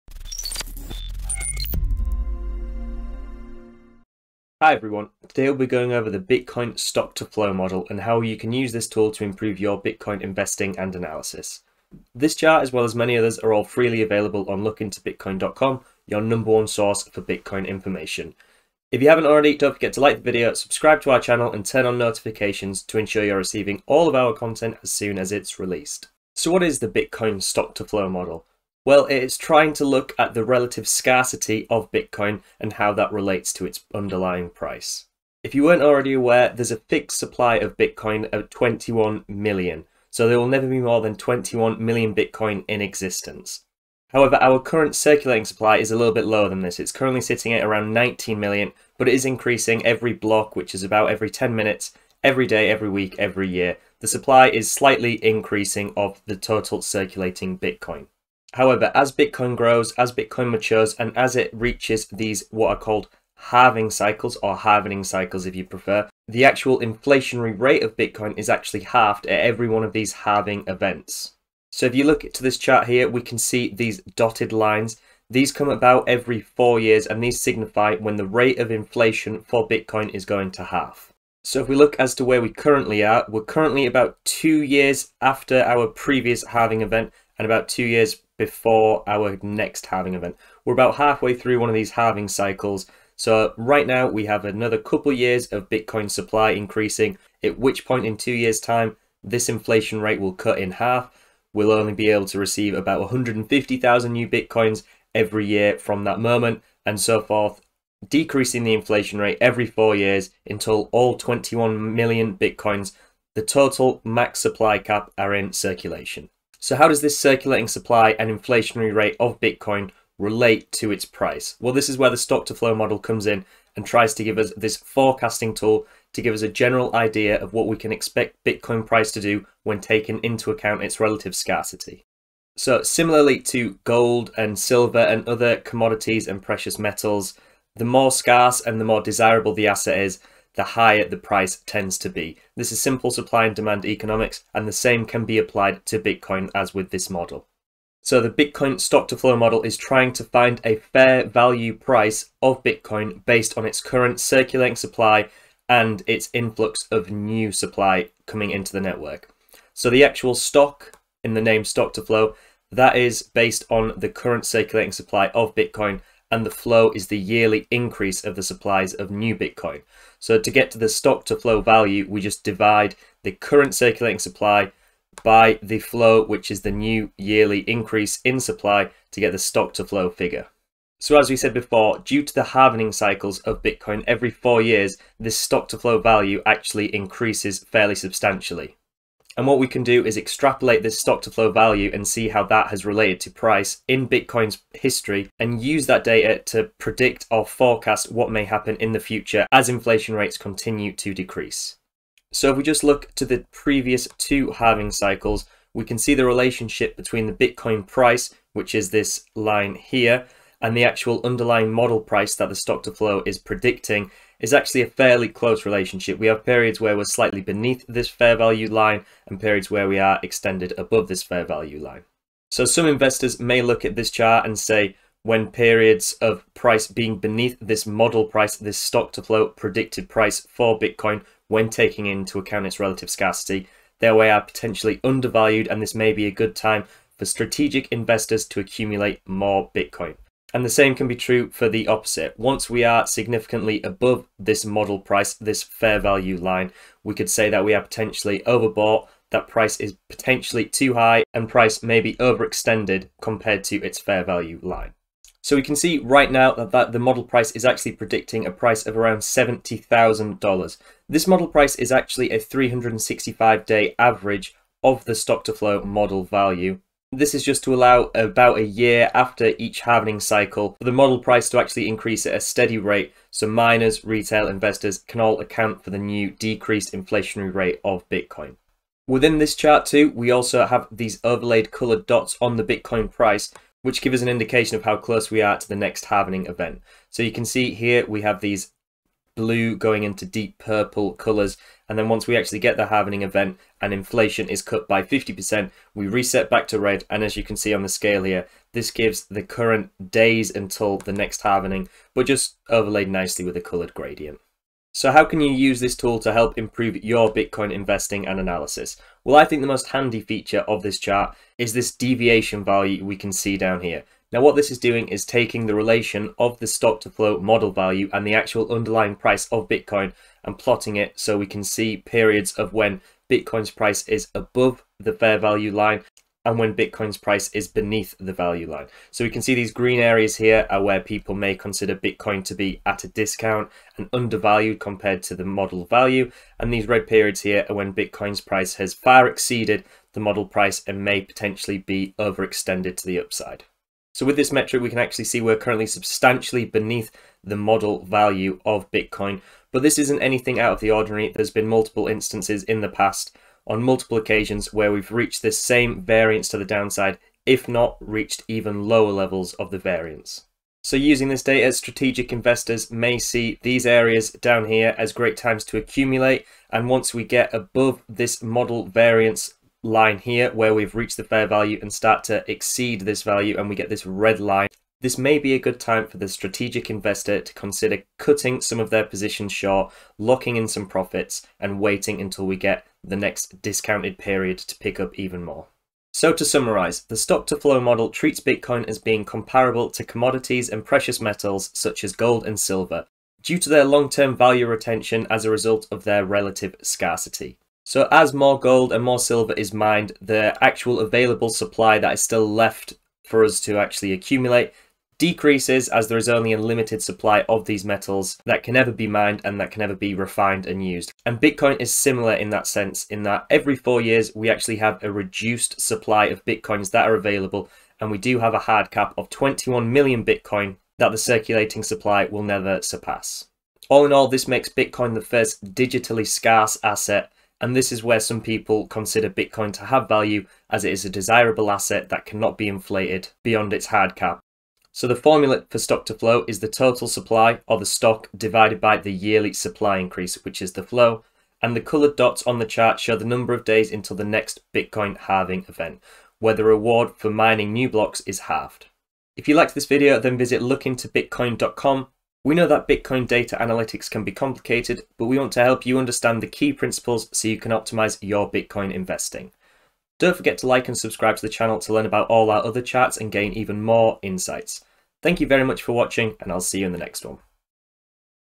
Stock-to-Flow Model
The stock-to-flow model treats Bitcoin as being comparable to commodities and precious metals such as gold, silver, or platinum. These are known as 'store of value' commodities because they retain value over long time frames due to their relative scarcity.

It is difficult to significantly increase their supply. For example, the process of searching for gold and then mining it is expensive and requires time and resources.

Bitcoin is similar because it is also scarce. In fact, it is the first-ever scarce digital object to exist! There are a limited number of coins in existence and it will take a lot of electricity and computing effort to mine the 2 million outstanding coins still to be mined. Therefore the supply rate is consistently low.

It is useful because it can be used as a forecasting tool to identify where the price of Bitcoin may go in the future. There are many other indicators that also forecast the price of Bitcoin, this particular tool focuses on the supply schedule of Bitcoin.